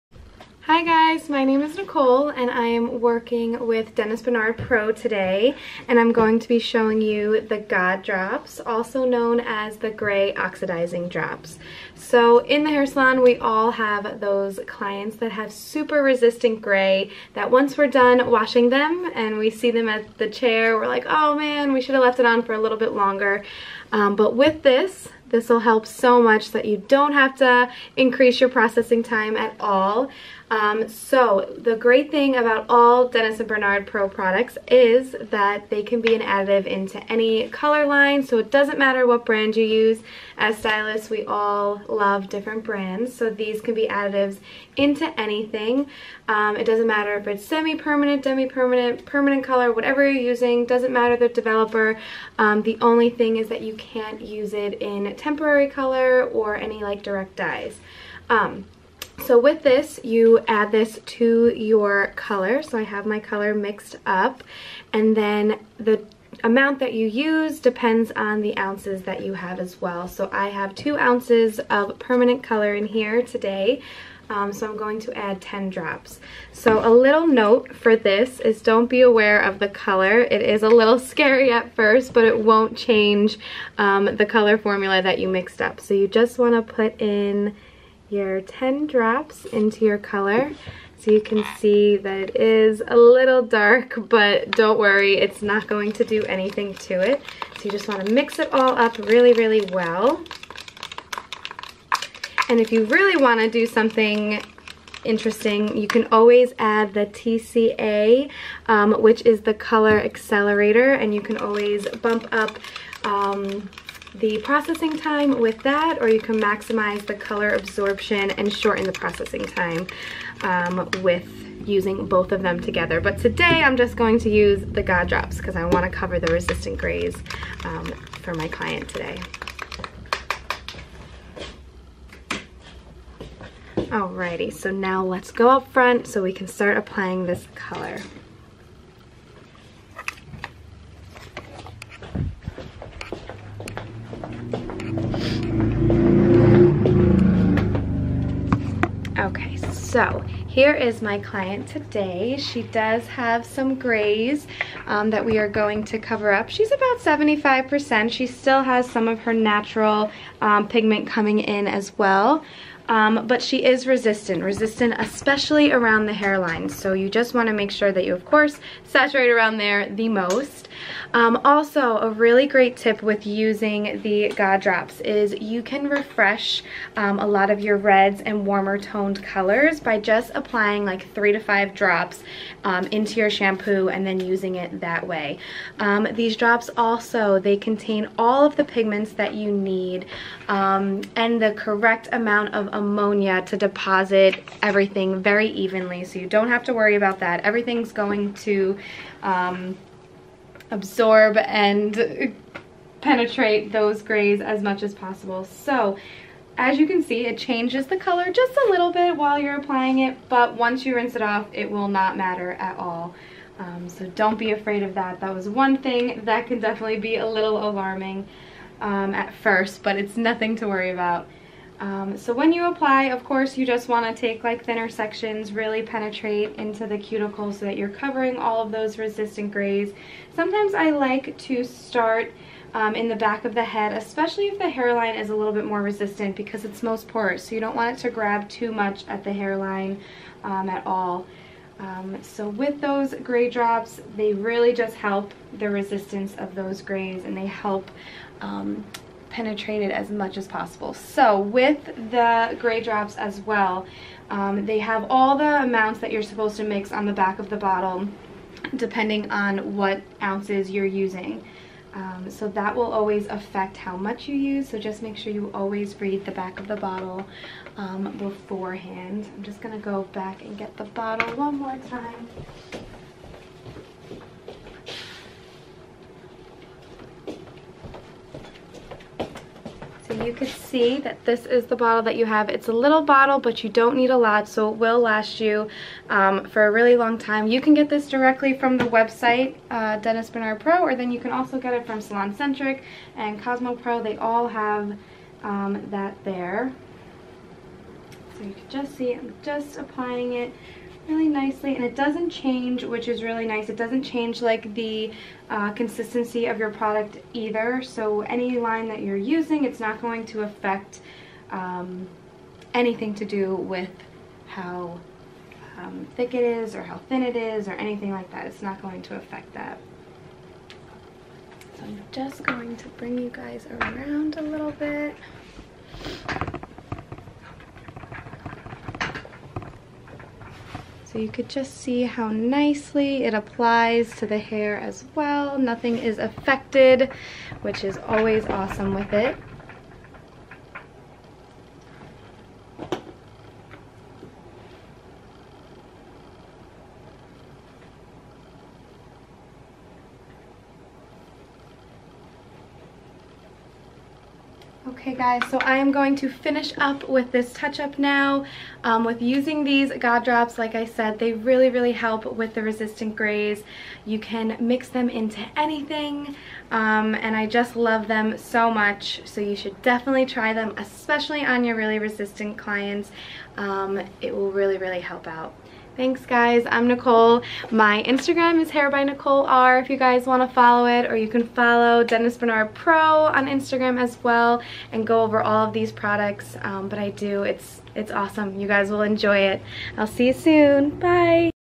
Powertools Gray Oxidizing Drops (GOD) - tutorial - by Dennis Bernard Powertools from MIX Ltd
Powertools Gray Oxidizing Drops (GOD) is a formula to add depth and shine to any manufacturers hair colour without affecting the processing time.

Permanent, demi and semi-permanent colour performances are enhanced when used and the hair colour is intensified... especially red hair.

Mix GOD and TCA (The Color Accelerator) into the colour mixture to achieve maximum colour absorption with shortened processing.

Powertools Gray Oxidizing Drops are compatible with all shades of colour and all POWERTOOLS products therefore a popular addition.